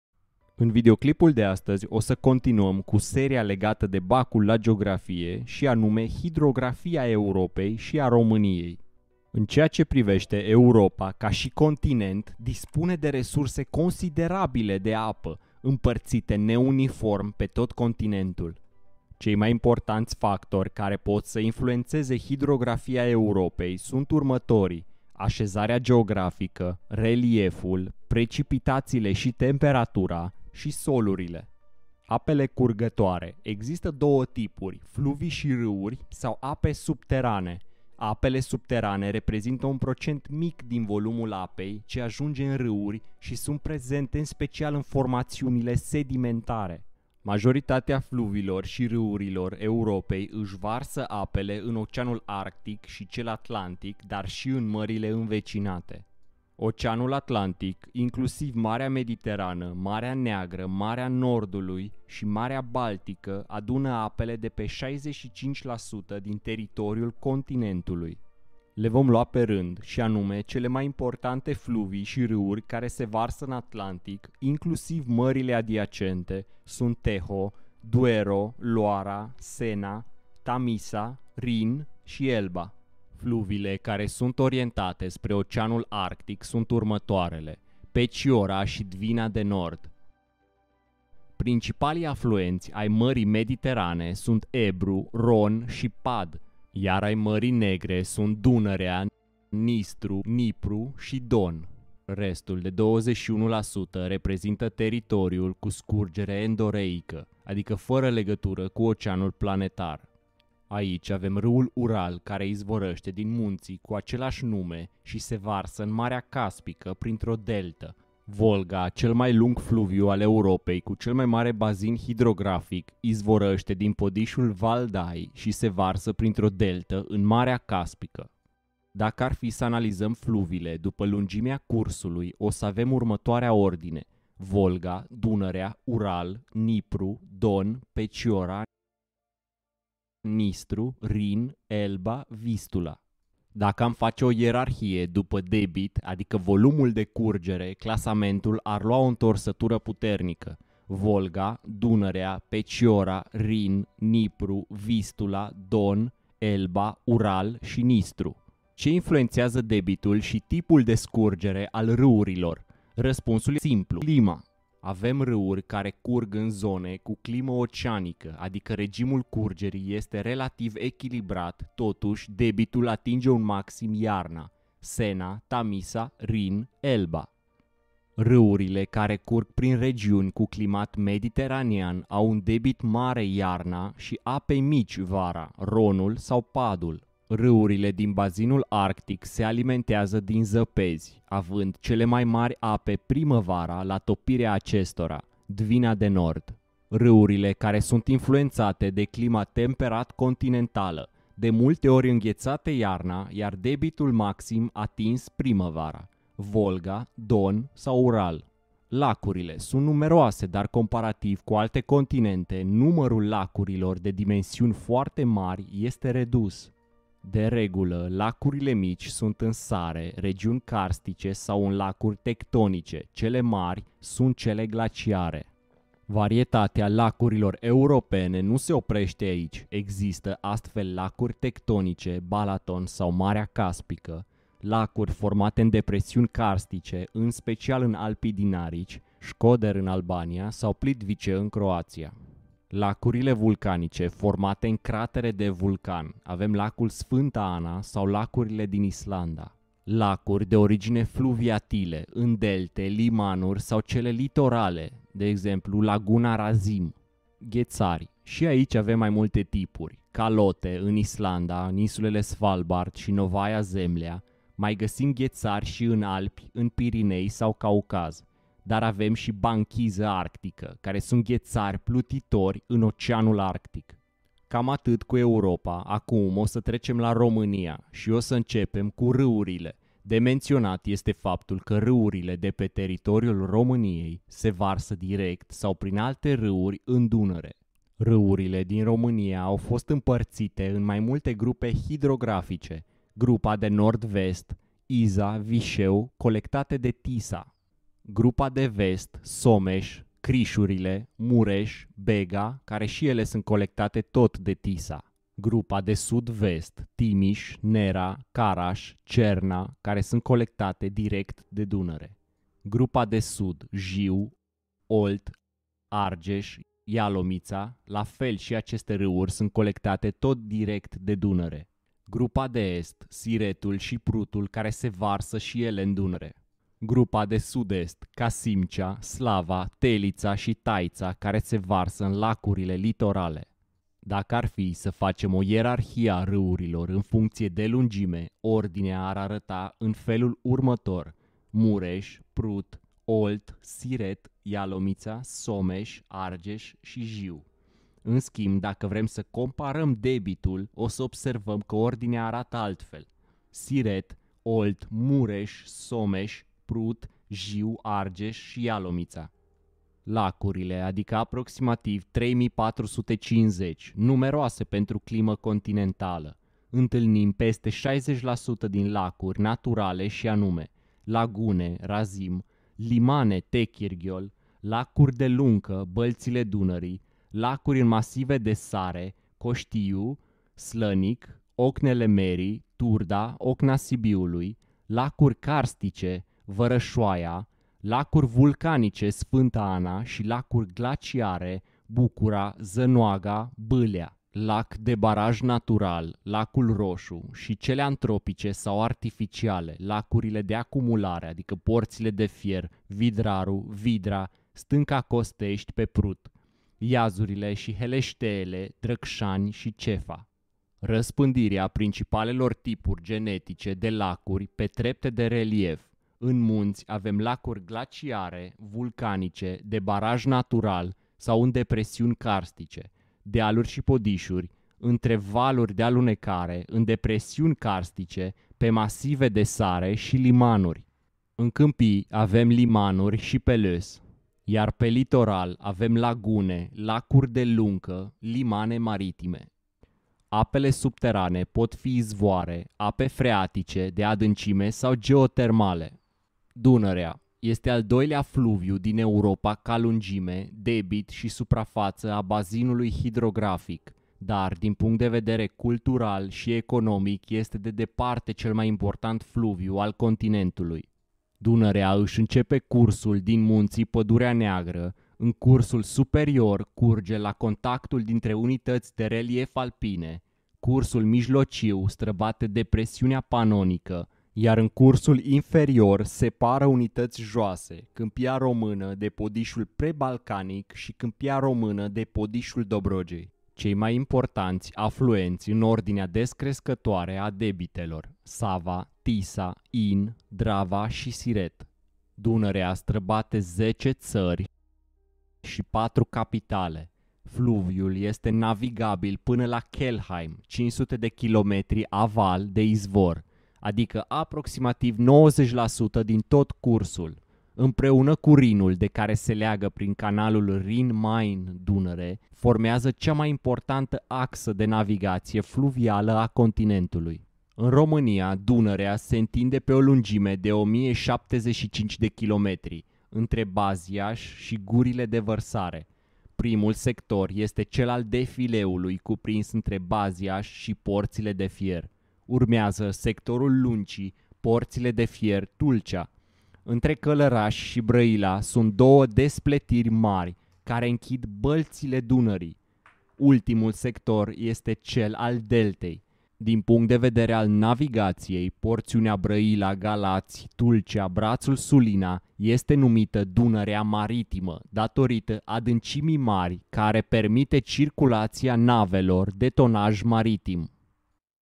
BAC GEOGRAFIE: Hidrografie
►Lectia/scenariul clipului il gasiti in primul comenariu! :D

Capitole:
0:00 - 3:30 Fluvii si Raurile Europei
3:30 - 4:39 Lacurile Europei
4:39 - 6:01 Raurile Romaniei
6:01 - 7:26 Lacurile Romaniei
7:26 - 9:21 Dunarea